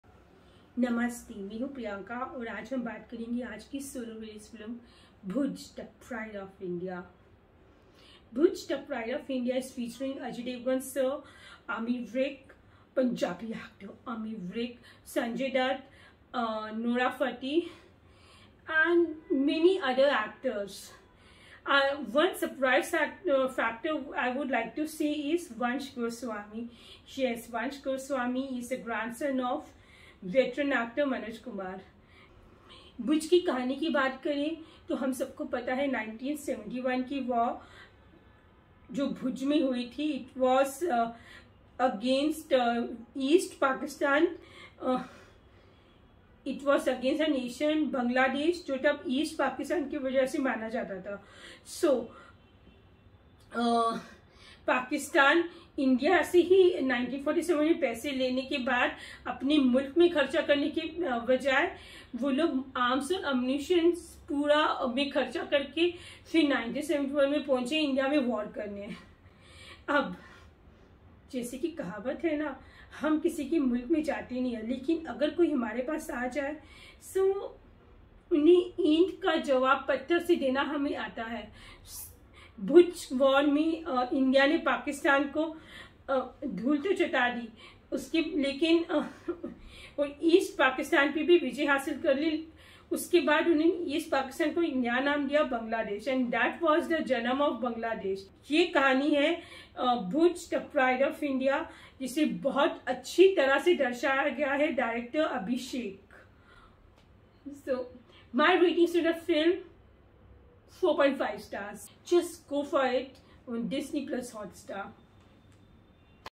BHUJ - The Pride of India (Review) By Priyanka Raina
BhujThePrideOfIndia featuring AjayDevgn , Sanjay Dutt, Nora Fateh, Ammy Virk , Vansh Goswami and many other actors.

(Review by PriyankaRaina)

Bhuj is a story of war that happened in the year 1971 , where Pakistan attacked from Bhuj in order to capture East Pakistan.  Which India won and Hence a new nation was formed "Bangladesh".

Dialogues of Bhuj are Thundering and deserves a clap.
Where as songs don't take attention.
Overall movie deserves a watch